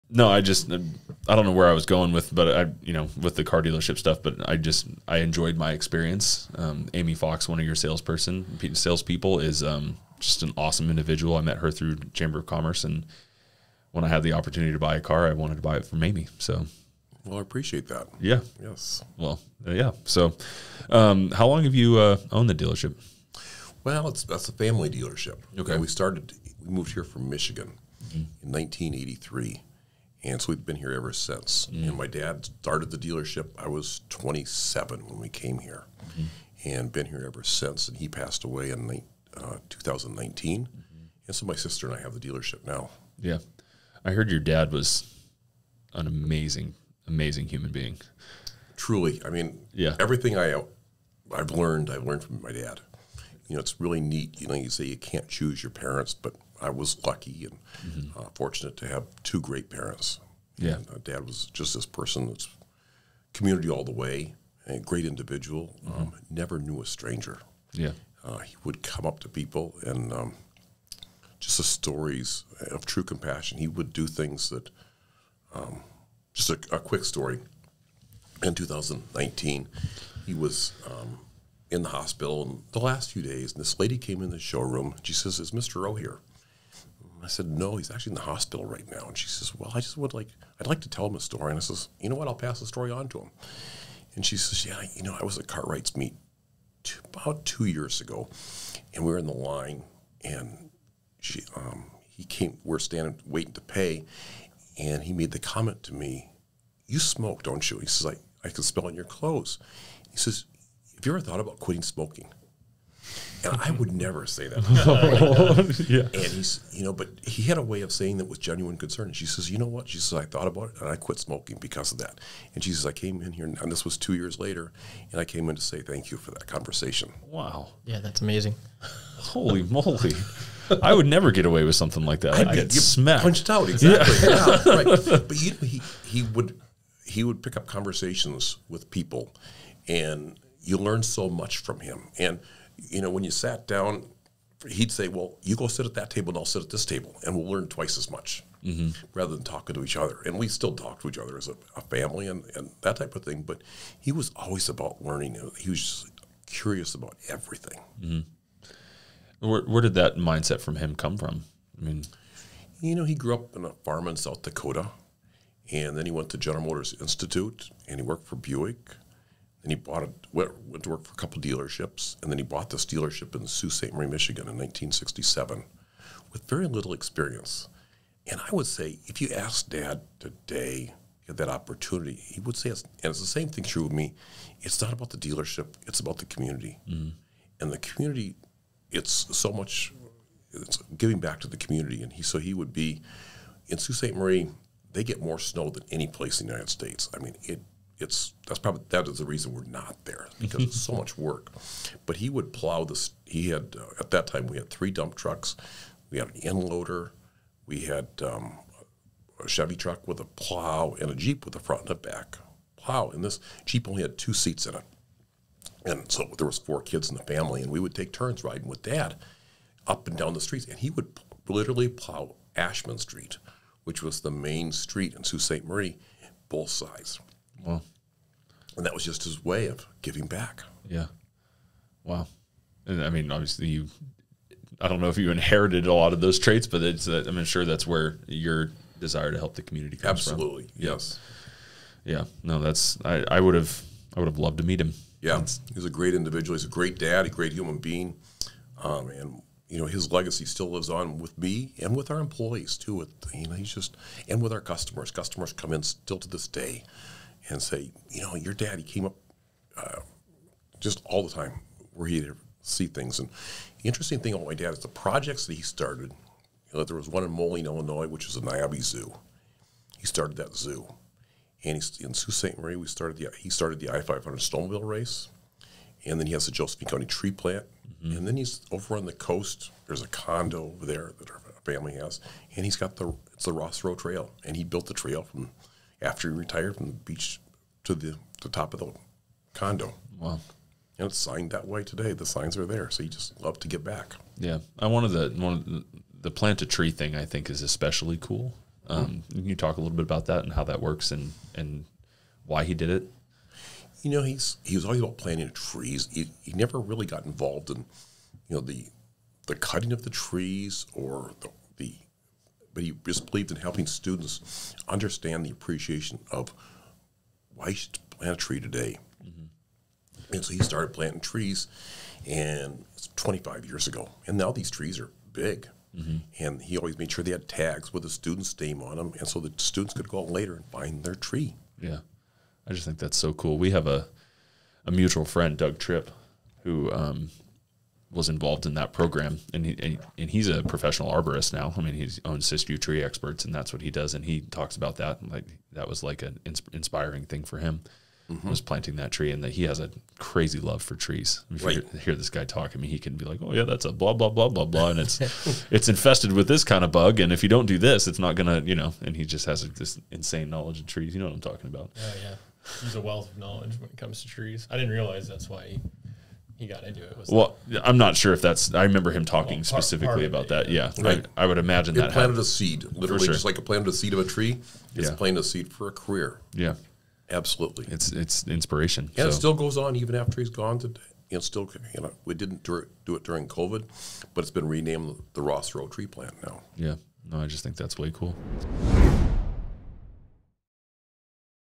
The Ross Roe Legacy - How Ross Roe left an Impact on Grants Pass and the Entire Rogue Valley
In this powerful episode, we dive into the impact that Steves father (Mr. Ross Roe) had not only on him and his family, but on our Grants Pass community.

In 1983, the Roe family sold their Michigan car dealership, moved across the country and started Roe Motors in Grants Pass, Oregon. In 1990, Mr. Roe started the Josephine County Tree Plant where over 23,000 youth planted over 100,000 trees on Josephine County land.  Mr. Roe also wrote the monthly back page of the Sneak Preview with articles about local events, a tradition that Steve now continues. At their Brookings, Oregon condo, Mr. Roe made a trail to the ocean for condo residents which is now known as the "Ross Roe Trail."  Mr. Roe volunteered in many community organizations such as the Bethany Presbyterian Church choir, Rogue Valley Symphony, and the RASCALS/Computer Bugs. His tireless energy and enthusiasm for the community was well-known in every community he lived in and Steve continues thats legacy today.

Check out this and the full episode here on YouTube or Listen on Spotify!